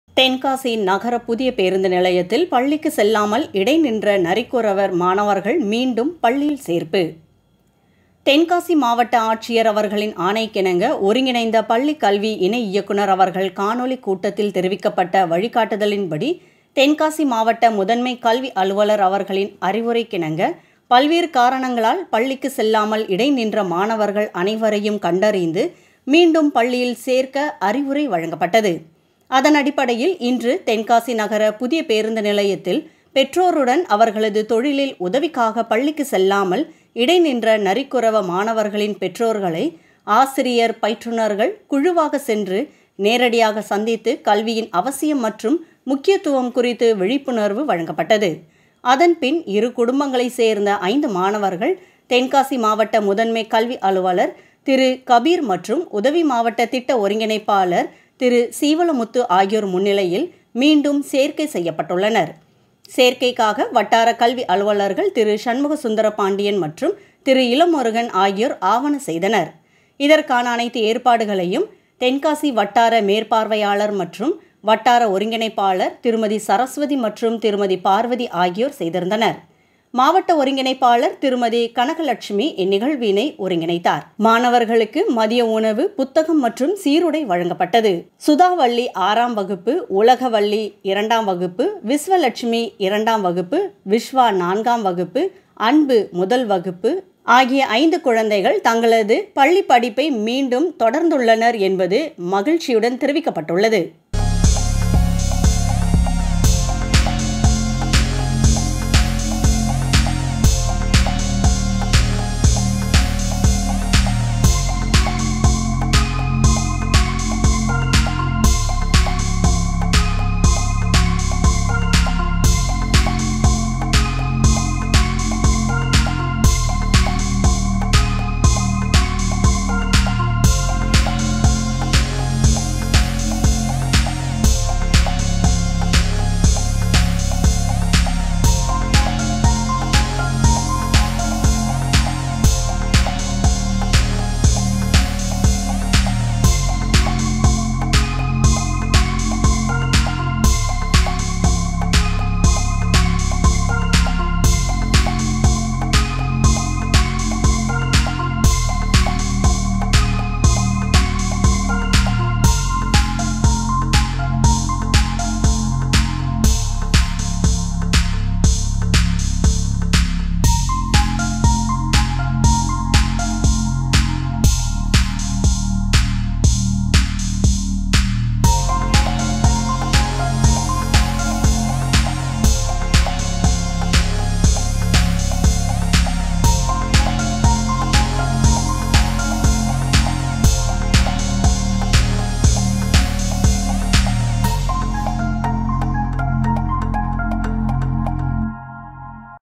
நரிக்குறவ மாணவர்கள் மீண்டும் பள்ளியில் சேர்ப்பு:
தென்காசி நகர புதிய பேருந்து நிலையத்தில் பள்ளிக்கு செல்லாமல் இடைநின்ற நரிக்குறவ மாணவர்கள் மீண்டும் பள்ளியில் சேர்ப்பு:

தென்காசி மாவட்ட ஆட்சியர் அவர்களின் ஆணைக்கிணங்க, ஒருங்கிணைந்த பள்ளி கல்வி இணை இயக்குனர் அவர்கள் காணொளிக் கூட்டத்தில் தெரிவிக்கப்பட்ட வழிகாட்டுதலின்படி, தென்காசி மாவட்ட முதன்மைக்கல்வி அலுவலர் அவர்களின்   அறிவுரைக்கிணங்க, பல்வேறு காரணங்களால் பள்ளிக்கு செல்லாமல் இடைநின்ற மாணவர்கள் அனைவரையும் கண்டறிந்து மீண்டும் பள்ளியில் சேர்க்க அறிவுரை வழங்கப்பட்டது.

அதனடிப்படையில் இன்று  தென்காசி நகர புதிய பேருந்து நிலையத்தில் பெற்றோருடன் அவர்களது தொழிலில் உதவிக்காக பள்ளிக்கு செல்லாமல் இடைநின்ற நரிக்குறவ மாணவர்களின் பெற்றோர்களை ஆசிரியர் பயிற்றுநர்கள்   குழுவாகச் சென்று நேரடியாக சந்தித்து கல்வியின் அவசியம் மற்றும் முக்கியத்துவம் குறித்து விழிப்புணர்வு வழங்கப்பட்டது. அதன்பின், இரு குடும்பங்களை சேர்ந்த ஐந்து மாணவர்கள்  தென்காசி மாவட்ட முதன்மை கல்வி அலுவலர், திரு. மு.கபீர் மற்றும் உதவி மாவட்ட திட்ட ஒருங்கிணைப்பாளர், திரு. சு. சீவல முத்து ஆகியோர் முன்னிலையில் மீண்டும் சேர்க்கை செய்யப்பட்டுள்ளனர்.   சேர்க்கைக்காக வட்டார கல்வி அலுவலர்கள் திரு. சண்முக சுந்தர பாண்டியன் மற்றும் திரு. இளமுருகன் ஆகியோர் ஆவன  செய்தனர்.

இதற்கான ஏற்பாடுகளை தென்காசி வட்டார மேற்பார்வையாளர் மற்றும் வட்டார ஒருங்கிணைப்பாளர் திருமதி. சரஸ்வதி மற்றும் திருமதி. பார்வதி ஆகியோர் செய்திருந்தனர். மாவட்ட ஒருங்கிணைப்பாளர் திருமதி. கனகலட்சுமி இந்நிகழ்வினை ஒருங்கிணைத்தார்.

மாணவர்களுக்கு மதிய உணவு, புத்தகம் மற்றும் சீருடை வழங்கப்பட்டது.

மாணவர்களின் பெயர்கள்:
1.சுதாவல்லி -ஆறாம் வகுப்பு
2. உலக வல்லி - இரண்டாம் வகுப்பு  3.விஸ்வ லட்சுமி- இரண்டாம் வகுப்பு
 4.விஷ்வா- நான்காம் வகுப்பு
5. அன்பு -முதல் வகுப்பு